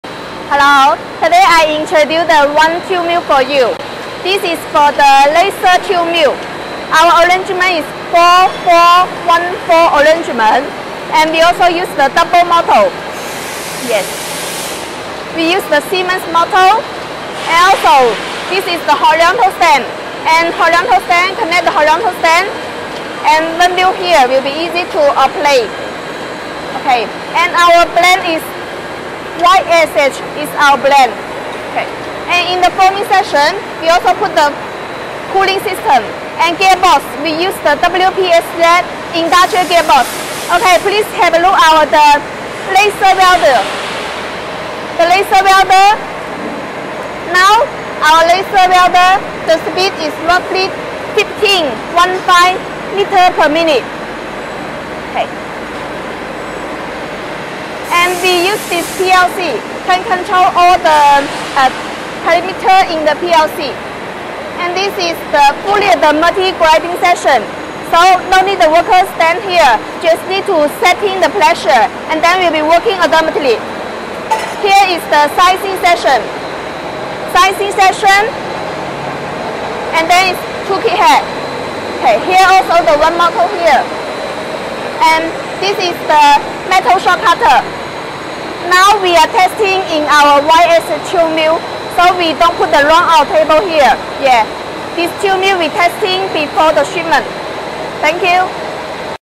Laser Welding Tube Mill | SS Pipe Making Machine
Laser welding tube mill is a machine designed to produce steel tubes with laser welding machine. The advantages about laser welding machine are 1.High Precision;2.Weld Seam more clean;3.Faster Welding;4.Higher Production Efficiency;5.Use electric welding, save welding cost.